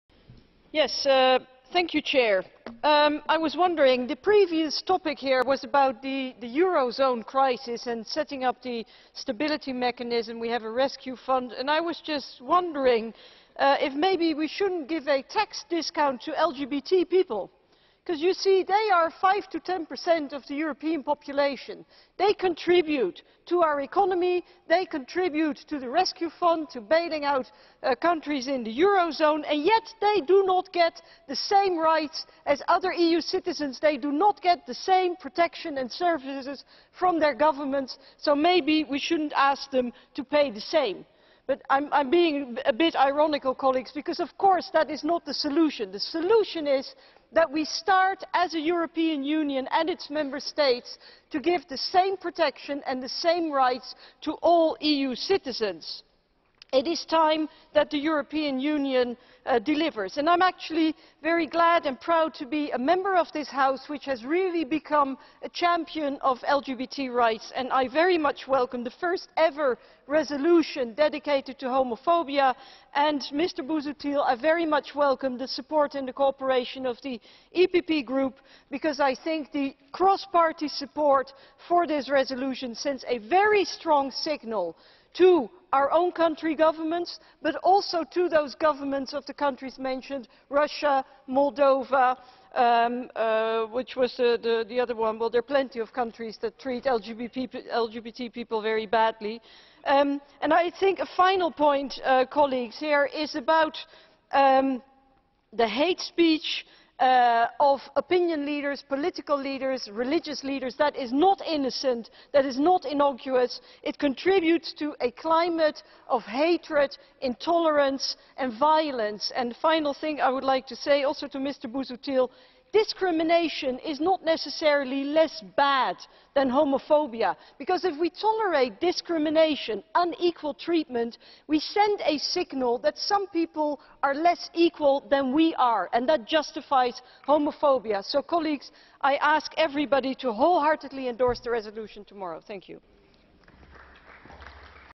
Sophia in 't Veld on Fight against homophobia in Europe
Speech by Sophia in 't Veld MEP (ALDE ADLE) on :
Fight against homophobia in Europe

in 't Veld-OR_120522_180233_obog_en